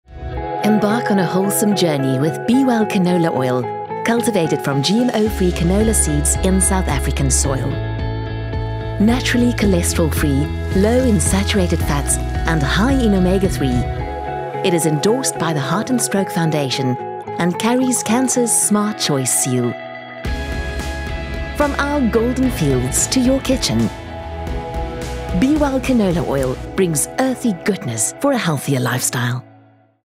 B-well Canola Oil - Proudly homegrown .
From our golden fields to your kitchen, our B-well Canola Oil is proudly homegrown, naturally cholesterol free and packed with omega-3. Endorsed by the Heart and Stroke Foundation South Africa and carrying the Cancer Association of South Africa's Smart Choice seal, there’s so many reasons for you to switchtocanola.

Visit us on bwellfoods.co.za to explore our range of ingredients for better living.